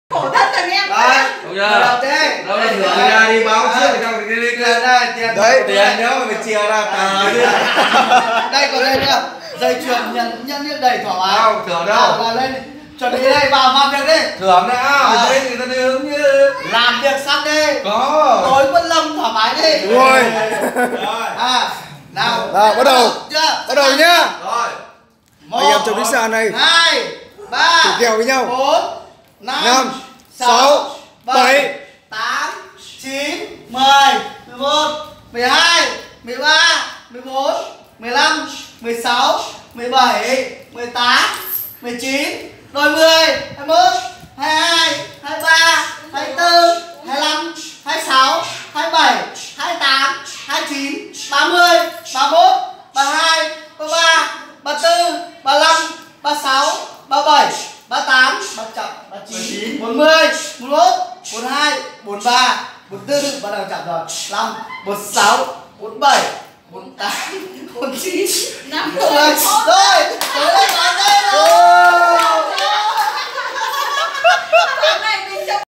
Anh em cá nhau 50 cái chống đẩy ai thắng được 1 triệu tối mua dê.
Mấy anh em rảnh rỗi đưa nhau cá cược vừa choi có tiền tối lại có dê ăn.Nhung cũng phải khen ông anh trong clip to béo thế mà chống đẩy được những 50 cái.Quá Tuyệt!!